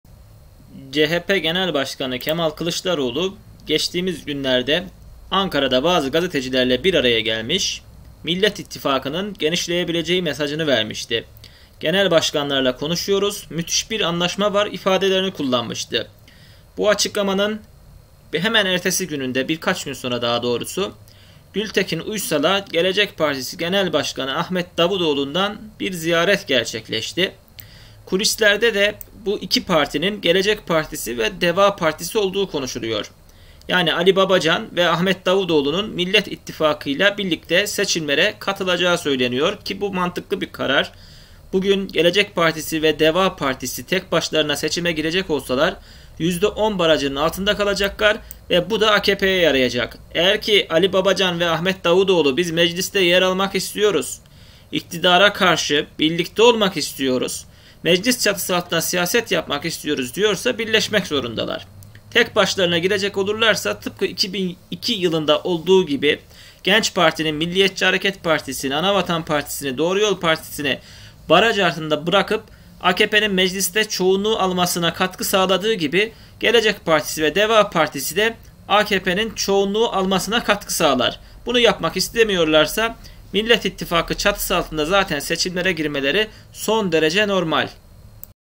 Millet İttifakına bomba katılım. Eski AKP'liler de geliyor
Millet İttifakı genişliyor. Ali Babacan liderliğindeki Deva Partisi ile Ahmet Davutoğlu liderliğindeki Gelecek Partisi Millet İttifakına katılacak.

Cumhuriyet gazetesinde yer alan habere göre: Gelecek Partisi ile Demokrat Parti’nin görüşmesi kulislerde daha önce ittifak dahilinde olmayan partilerin ‘Millet İttifakı’nda yer alabileceklerine’ ilişkin sinyal olduğu konuşuluyor.

İyi Parti genel başkanı Meral Akşener ve CHP Genel Başkanı Kemal Kılıçdaroğlu'da bu katılıma sıcak bakıyor.

CHP, İyi Parti, Saadet Partisi, Demokrat Parti, Gelecek Partisi ve Deva Partisi bu kapsamda seçime birlikte katılacaklar.

Aksi takdirde 2022 yılındaki Genç Parti ve Cem Uzan'ın meclis çoğunluğunu AKP'ye kaptırması gibi durum söz konusu olabilecek..